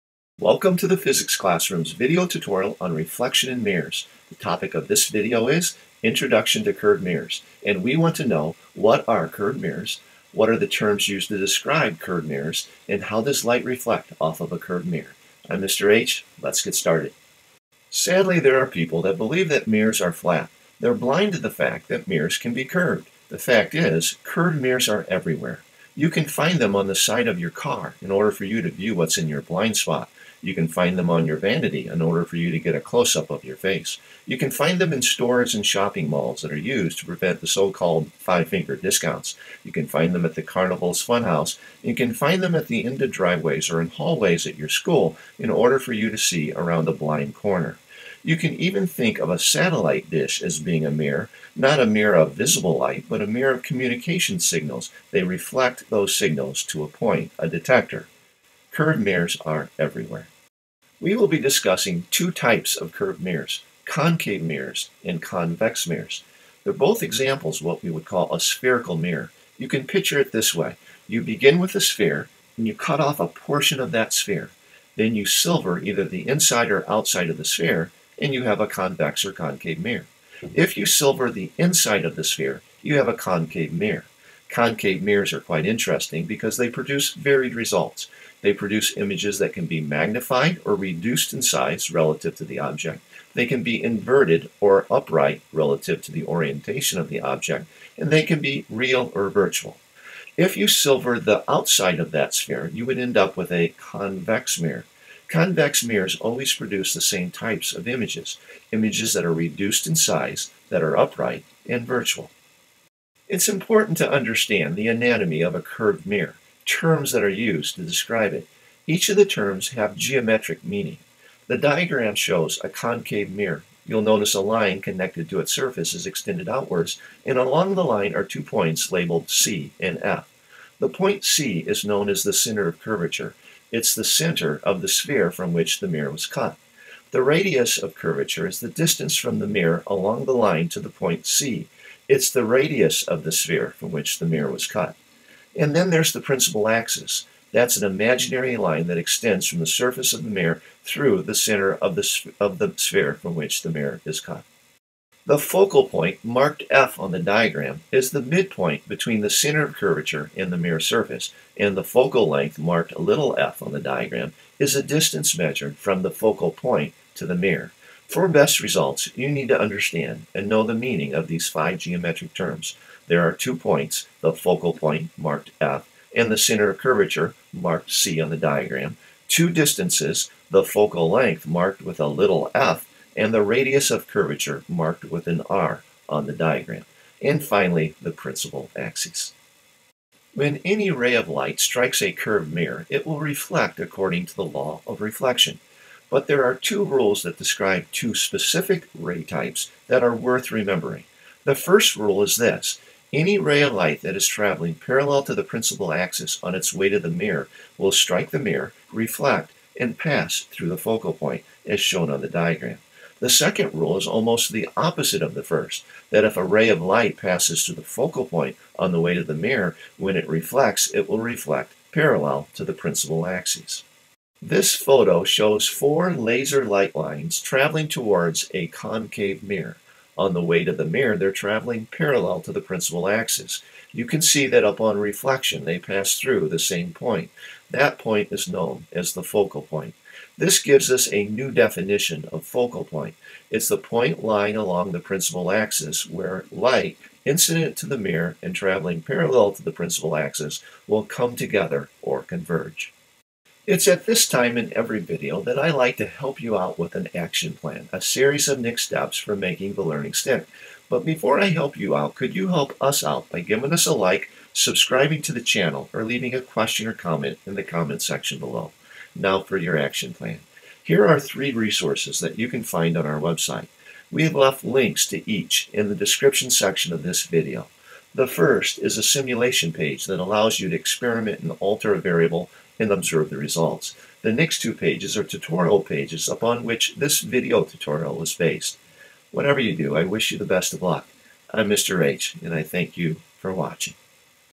Introduction to Curved Mirrors
In 5 snappy minutes, Mr. H explains what a spherical mirror is and identifies the two types (concave and convex), The terms associated with a curved mirror's "anatomy" are discussed. The manner in which light reflects off a curve mirror is explained.

Student Action Plan includes:
Physics Interactives, Reflection and Mirrors section, Optics Bench - Curved Mirrors

Tutorial on Reflection and the Ray Model of Light, Lesson 3: Anatomy of a Curved Mirror

Tutorial on Reflection and the Ray Model of Light, Lesson 3: Rules of Reflection for Concave Mirrors